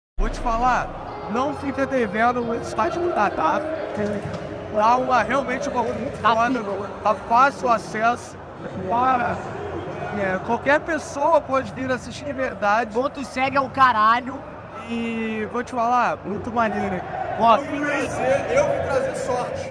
Igor 3K fala do 1° jogo do Galo na ARENA MRV no Flow Podcast
Veja o que o Igor 3K do Flow falou da inauguração da Arena MRV no jogo entre Atlético e Santos

Siga meu Instagram para participar dos sorteios do canal